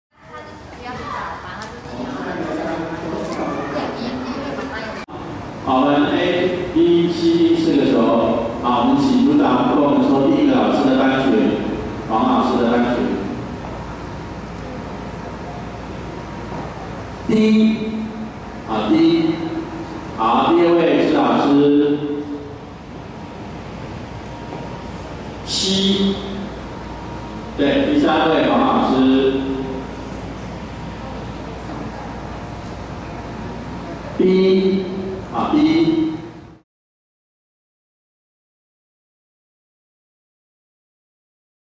看過來！彰化區國小新生編班結果出爐了！
出爐了！彰化縣彰化區、鹿港區、北斗區國小策略聯盟，今天舉行新生入學電腦編班作業，今年適逢龍子龍女入學，全縣小一新生人數10445人，比去年增加1155人，今年南郭解除總額管制，南郭與中山各增2班，南郭小一共有16班，中山小一12班。